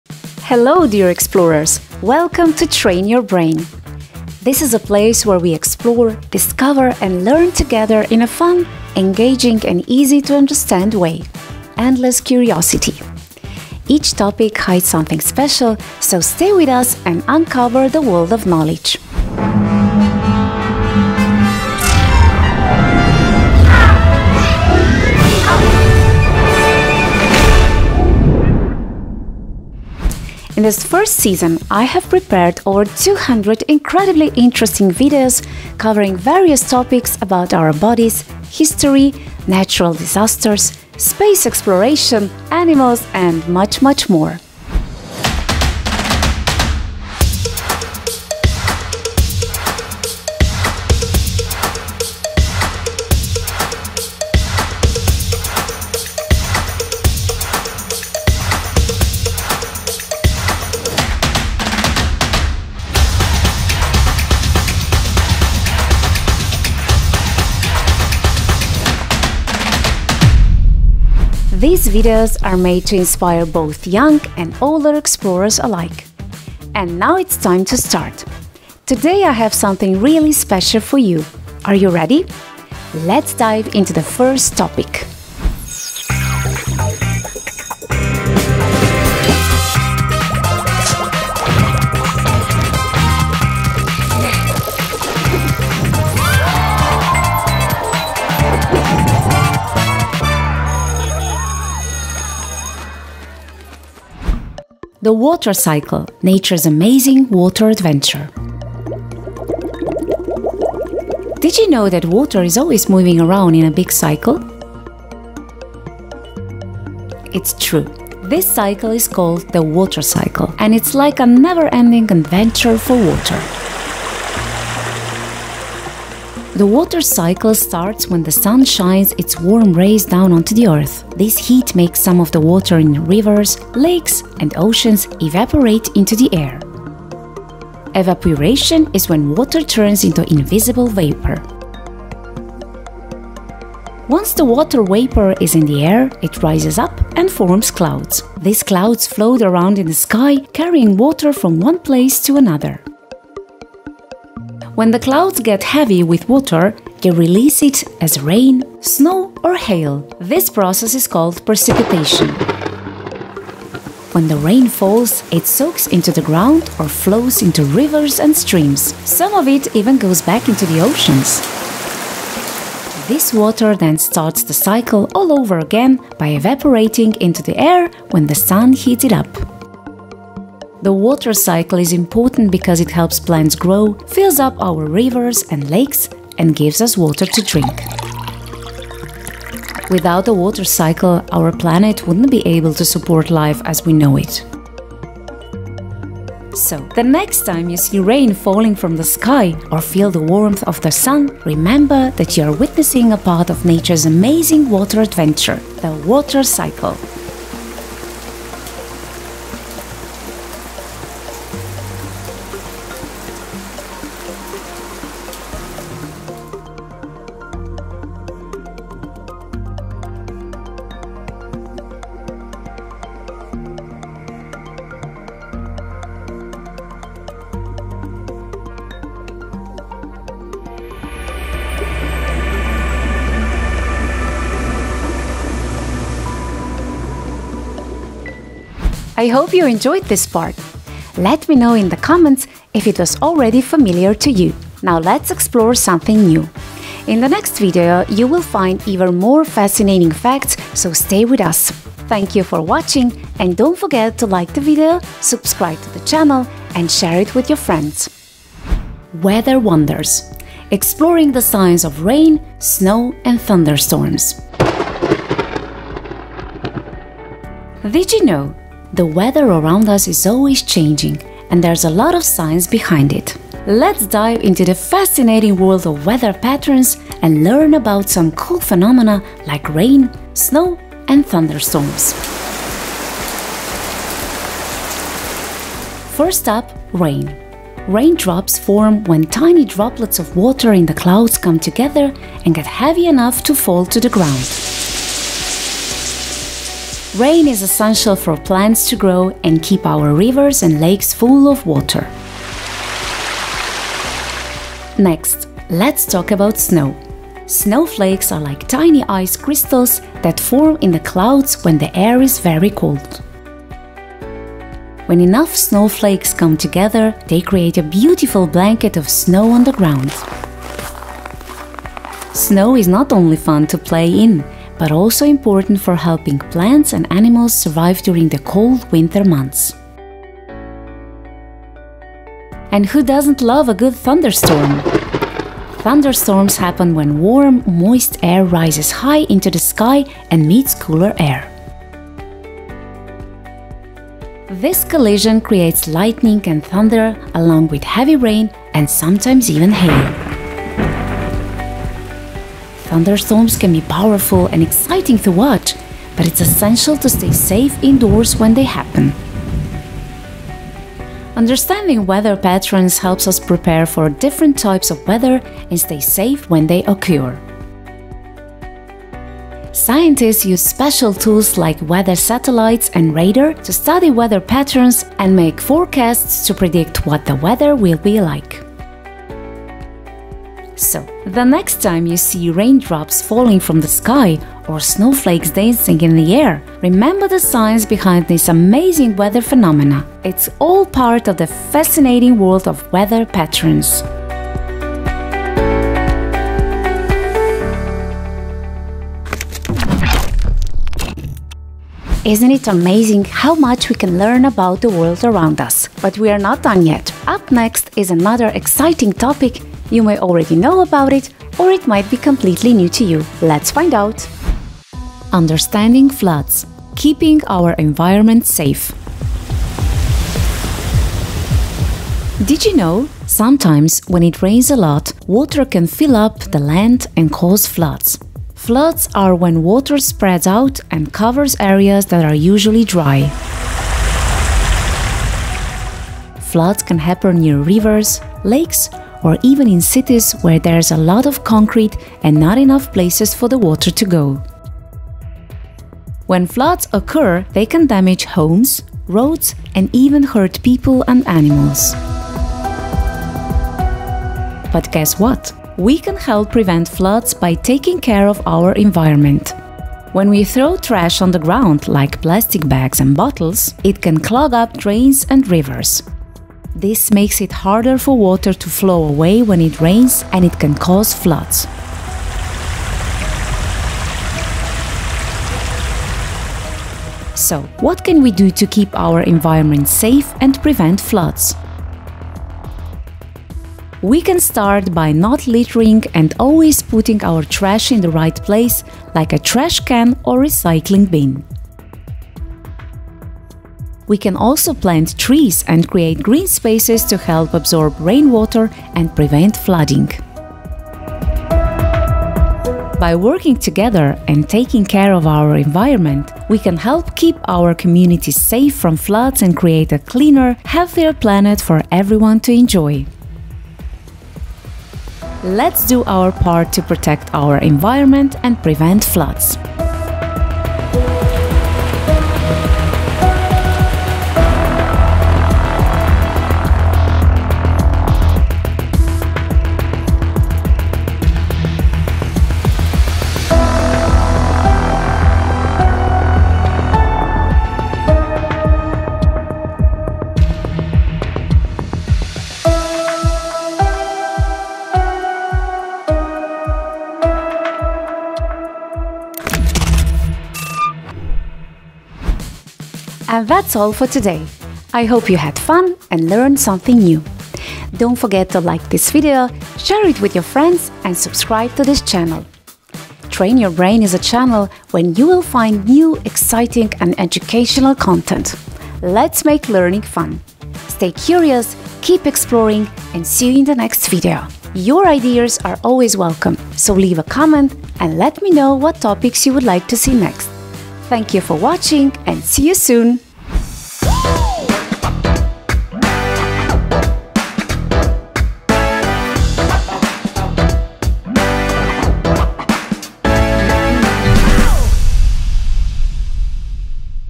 WATER CYCLE Explained 🌧️ From Cloud to Flood!
How does water move through nature? Why does it rain, snow, or flood?
In this episode of Train Your Brain IQ, we dive into the incredible water cycle and reveal how water transforms our environment!

📌 What you’ll learn:
01:44 – The Water Cycle 🌦️
04:19 – The Science of Rain, Snow & Storms ⚡❄️
06:55 – Understanding Floods and Staying Safe 🌊

This video is perfect for kids, parents, and teachers looking to explore nature’s most powerful force — water!
💡 Fun, visual, and packed with science facts.

🔔 Don’t miss the next episode of Train Your Brain IQ!